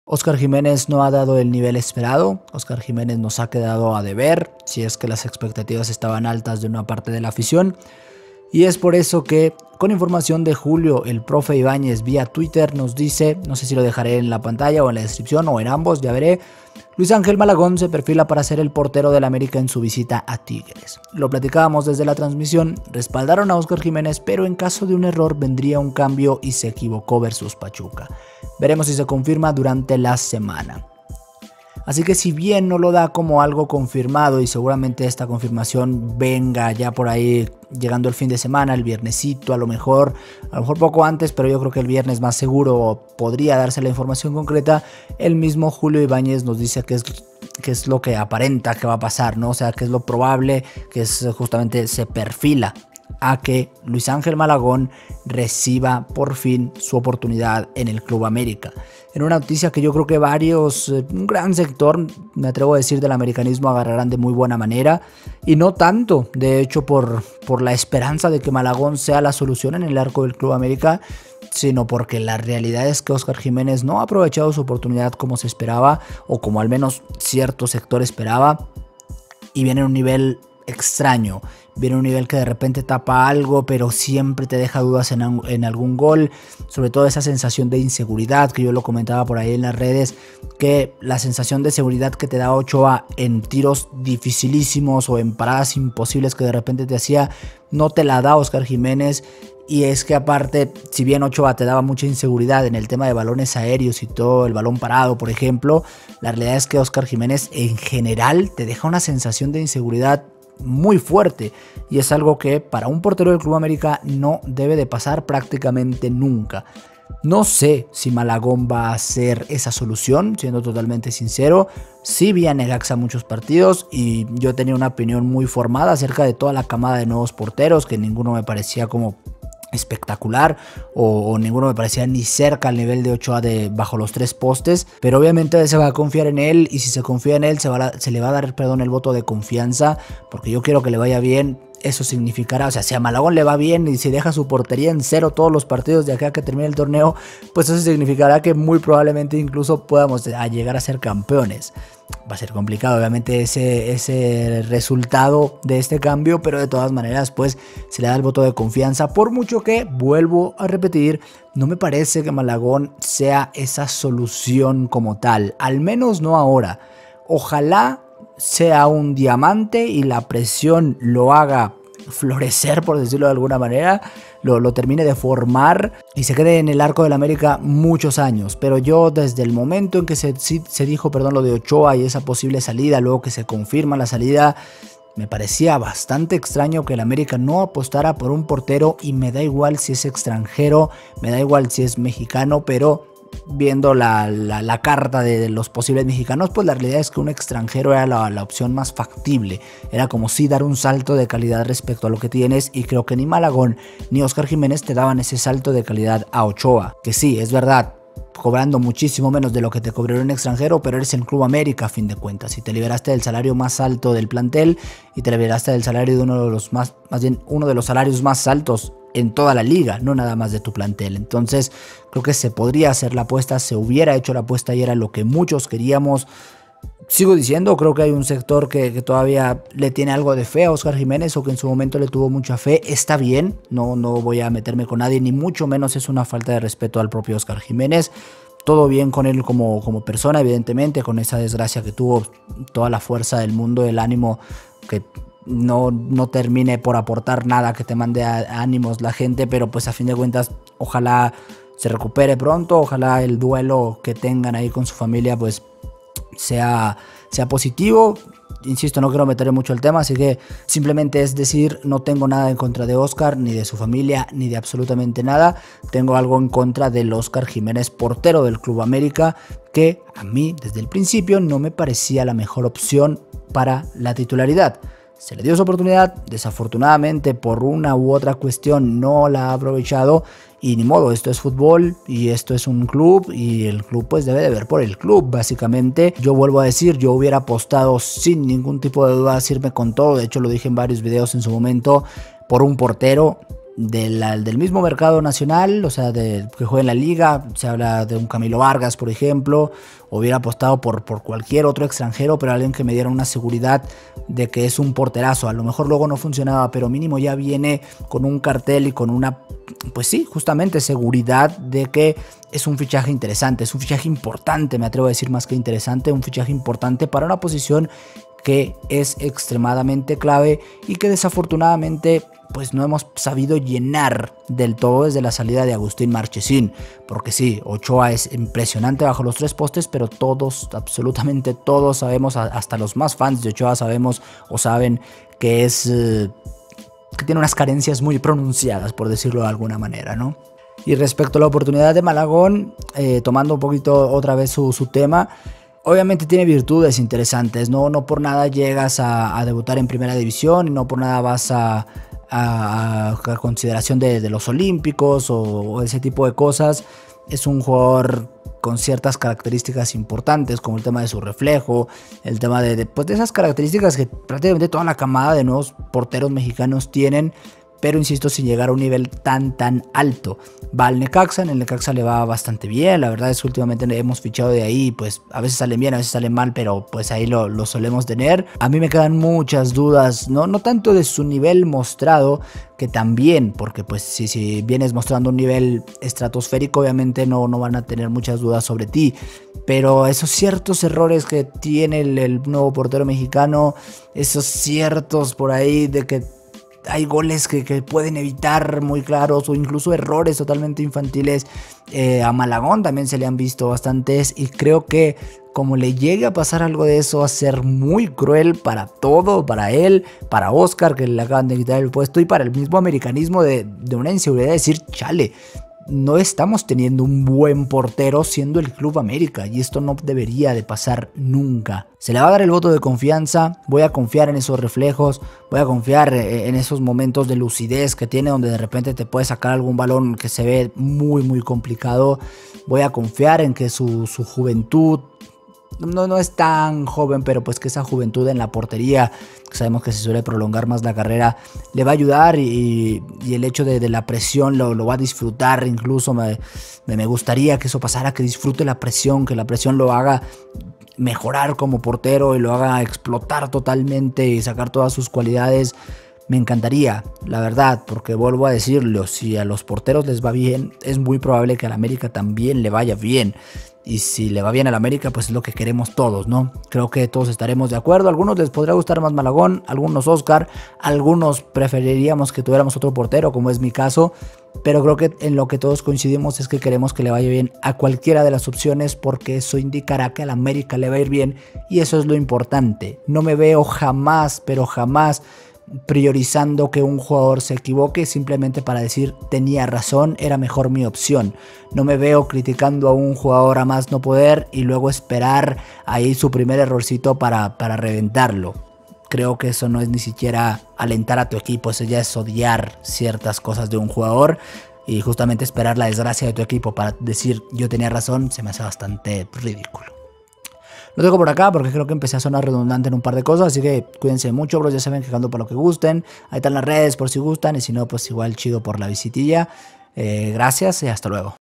Luis Ángel Malagón sería titular vs Tigres. Oscar Jiménez banca.
Luis Ángel Malagón se perfila para ser el portero del America en su visita a Tigres, lo platicábamos desde la transmisión, respaldaron a Óscar Jiménez pero en caso de un error vendría un cambio y se equivocó vs Pachuca

Veremos si se confirma durante la semana